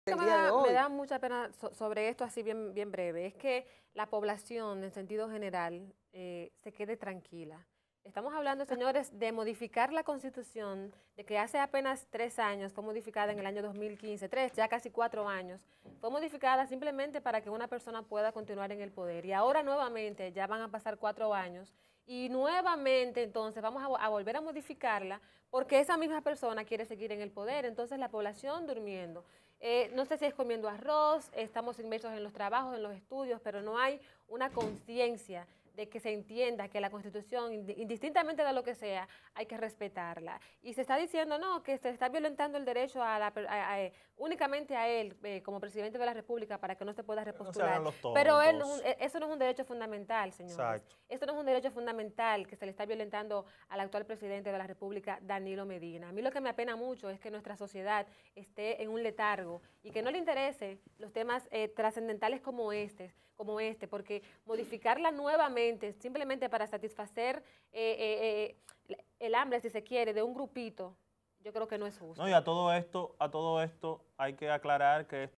La dominicanos están durmiendo con la reelección
La sociedad dominicana no despierta con un tema tan trascendental.